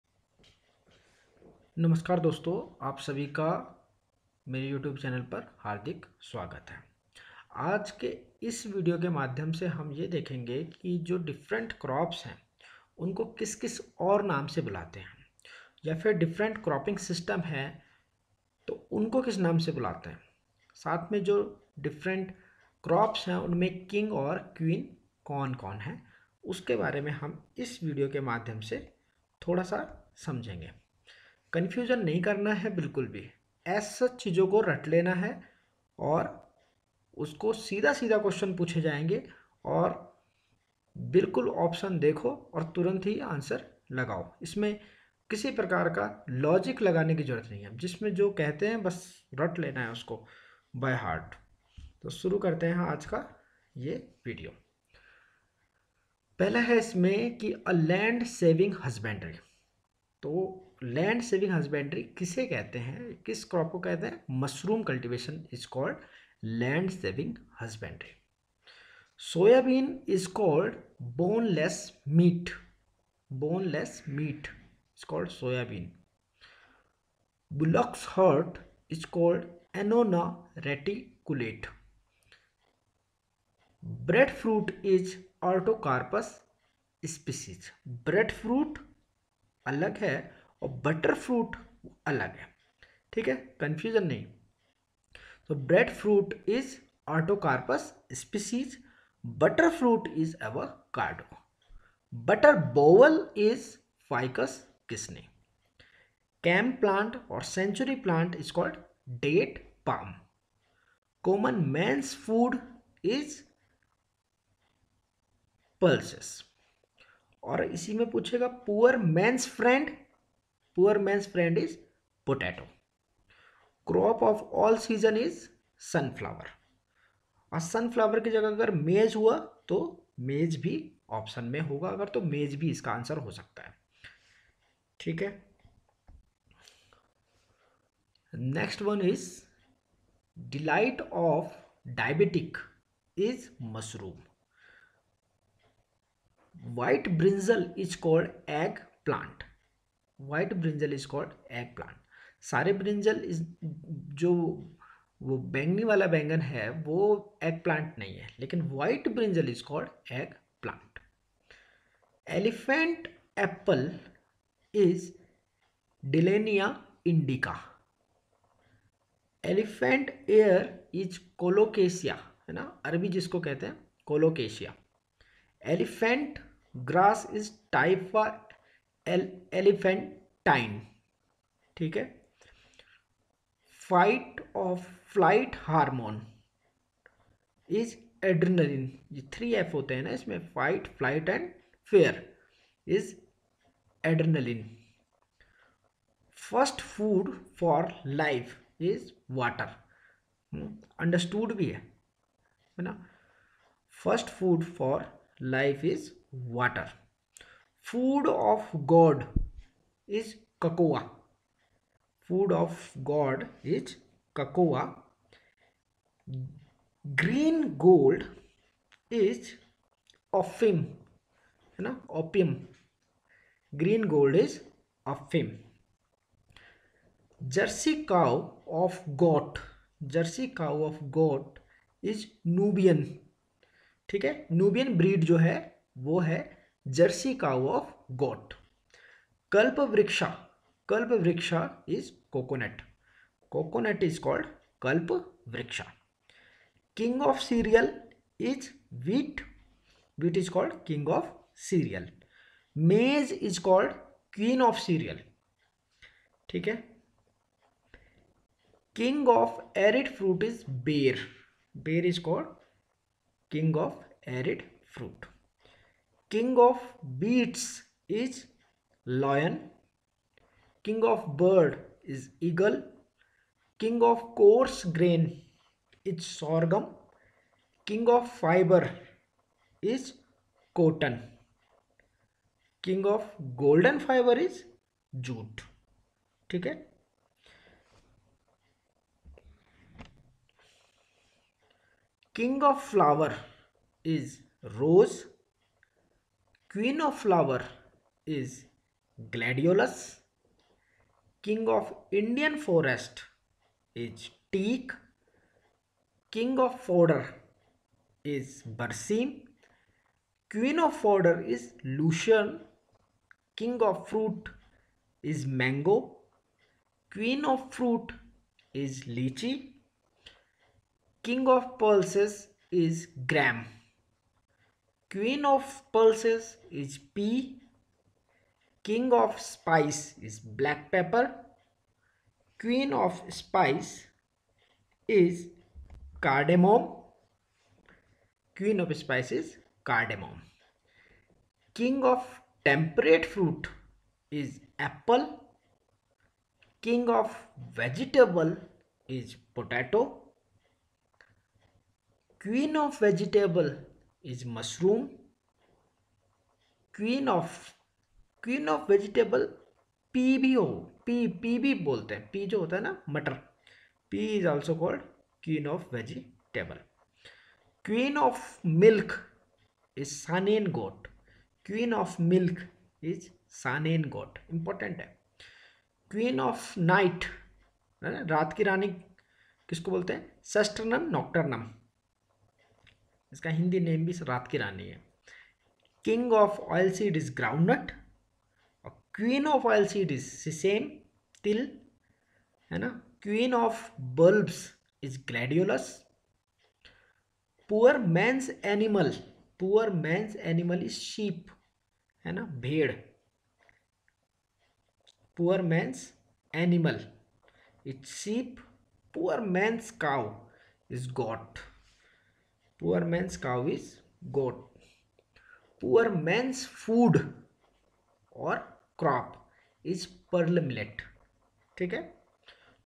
Some important point for agri exams
In this video, we will learn some common name of different crops like king and queen which is generally asked in different competetive exams.